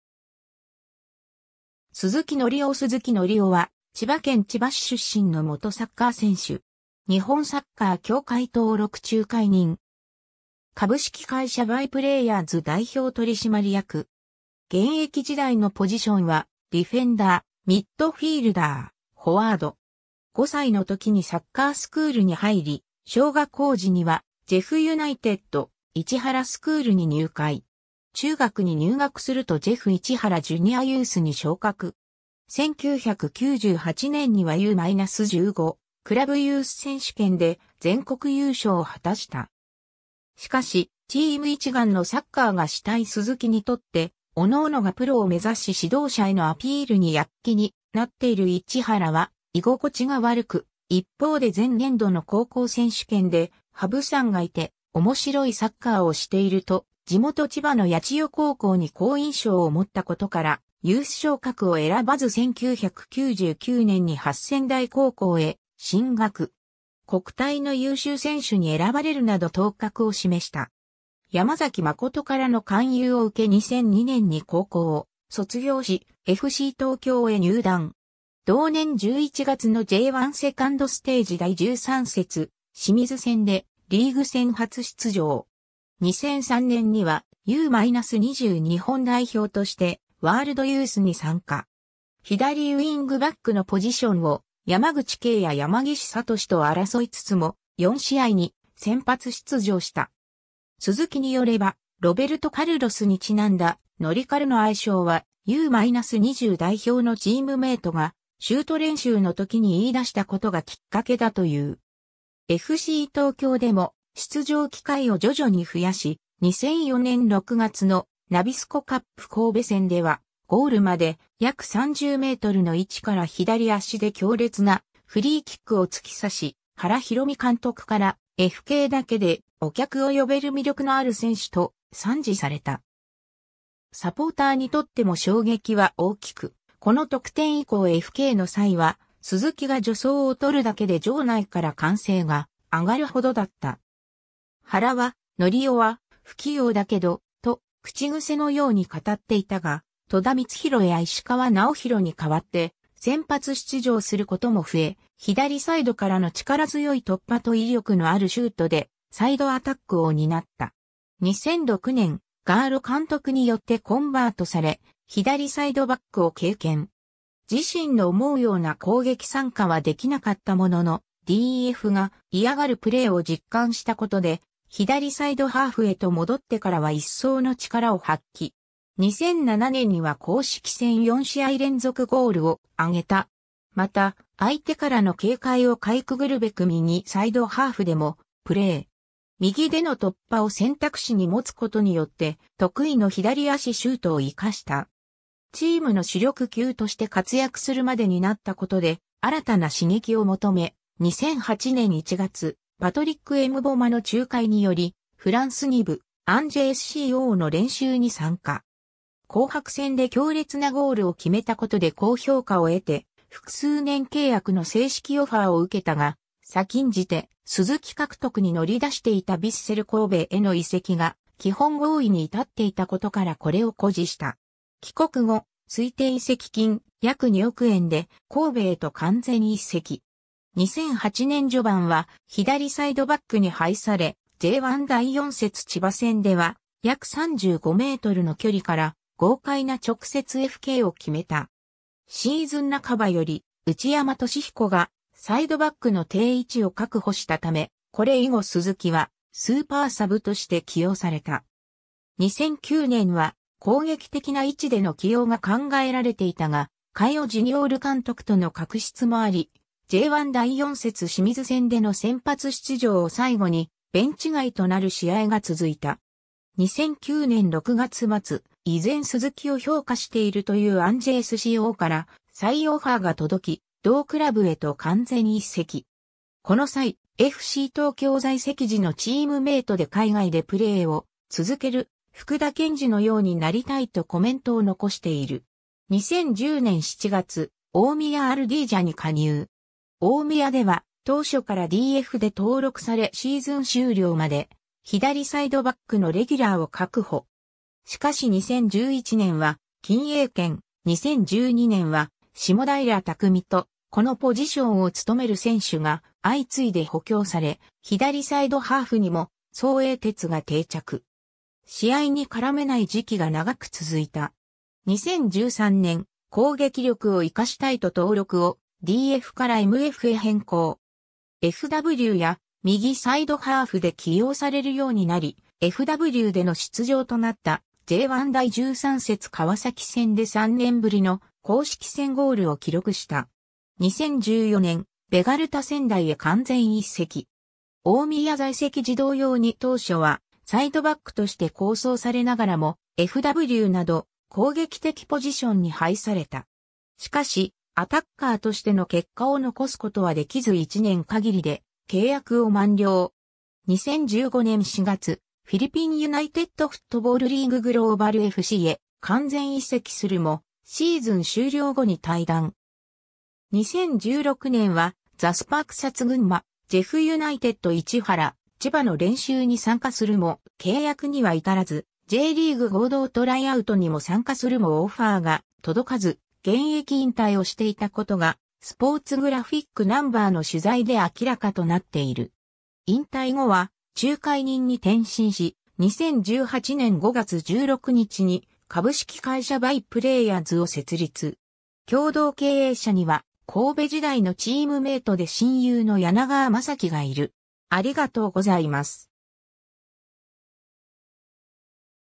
鈴木規郎
鈴木規郎 鈴木 規郎（すずき のりお、1984年2月14日 - ）は、千葉県千葉市出身の元サッカー選手。
日本サッカー協会登録仲介人。
株式会社By players代表取締役。
 現役時代のポジションはディフェンダー（左サイドバック）、ミッドフィールダー（左サイドハーフなど）、フォワード（ウイング）。
 5歳の時にサッカースクールに入り、小学校時にはジェフユナイテッド市原スクールに入会。
中学に入学するとジェフ市原ジュニアユースに昇格。
1998年にはU-15クラブユース選手権で全国優勝を果たした。
しかし、チーム一丸のサッカーがしたい鈴木にとって各々がプロを目指し指導者へのアピールに躍起になっている市原は居心地が悪く、一方で前年度の高校選手権で「羽生（直剛）さんがいて、面白いサッカーをしている」と地元千葉の八千代高校に好印象を持ったことから、ユース昇格を選ばず1999年に八千代高校へ進学。
国体の優秀選手に選ばれるなど頭角を示した。
 山崎真からの勧誘を受け 2002年に高校を卒業し、FC東京へ入団。
同年11月のJ1セカンドステージ第13節清水戦でリーグ戦初出場。
2003年にはU-20日本代表としてワールドユースに参加。
左ウイングバックのポジションを山口慶や山岸智と争いつつも、4試合に先発出場した。
鈴木によれば、ロベルト・カルロスにちなんだ「ノリカル」の愛称は、U-20代表のチームメートがシュート練習の時に言い出したことがきっかけだという。
 FC東京でも出場機会を徐々に増やし、2004年6月のナビスコカップ神戸戦では、ゴールまで約30メートルの位置から左足で強烈なフリーキックを突き刺し、原博実監督から「FKだけでお客を呼べる魅力のある選手」と賛辞された。
サポーターにとっても衝撃は大きく、この得点以降FKの際は、鈴木が助走を取るだけで場内から歓声が挙がる程だった。
原は「規郎は不器用だけど…」と口癖のように語っていたが、戸田光洋や石川直宏に代わって先発出場することも増え、左サイドからの力強い突破と威力のあるシュートでサイドアタックを担った。
 2006年、ガーロ監督によってコンバートされ、左サイドバックを経験。
自身の思うような攻撃参加はできなかったものの、DFが嫌がるプレーを実感したことで、左サイドハーフへと戻ってからは一層の力を発揮。
2007年には公式戦4試合連続ゴールを挙げた。
また、相手からの警戒を掻い潜るべく右サイドハーフでもプレー。
右での突破を選択肢に持つことによって得意の左足シュートを活かした。
 チームの主力級として活躍するまでになったことで新たな刺激を求め、2008年1月、パトリック・エムボマの仲介によりフランス2部・アンジェSCOの練習に参加。
紅白戦で強烈なゴールを決めたことで高評価を得て、複数年契約の正式オファーを受けたが、先んじて鈴木獲得に乗り出していたヴィッセル神戸への移籍が基本合意に至っていたことからこれを固辞した。
 帰国後、推定移籍金約2億円で神戸へと完全移籍。
2008年序盤は左サイドバックに配され、J1第4節千葉戦では約35メートルの距離から豪快な直接FKを決めた。
シーズン半ばより内山俊彦がサイドバックの定位置を確保したため、これ以後鈴木はスーパーサブとして起用された。
2009年は攻撃的な位置での起用が考えられていたが、カイオ・ジュニオール監督との確執もあり、J1第4節清水戦での先発出場を最後にベンチ外となる試合が続いた。
 2009年6月末、依然鈴木を評価しているというアンジェSCOから再オファーが届き、同クラブへと完全移籍。
この際、FC東京在籍時のチームメートで海外でプレーを続ける福田健二のようになりたいとコメントを残している。
 2010年7月、大宮アルディージャに加入。
大宮では当初からDFで登録されシーズン終了まで左サイドバックのレギュラーを確保。
しかし2011年は金英權、2012年は下平匠と、このポジションを務める選手が相次いで補強され、左サイドハーフにも曺永哲が定着。
試合に絡めない時期が長く続いた。
2013年、攻撃力を活かしたいと登録をDFからMFへ変更。
FWや右サイドハーフで起用されるようになり、FWでの出場となったJ1第13節川崎戦で3年ぶりの公式戦ゴールを記録した。
 201...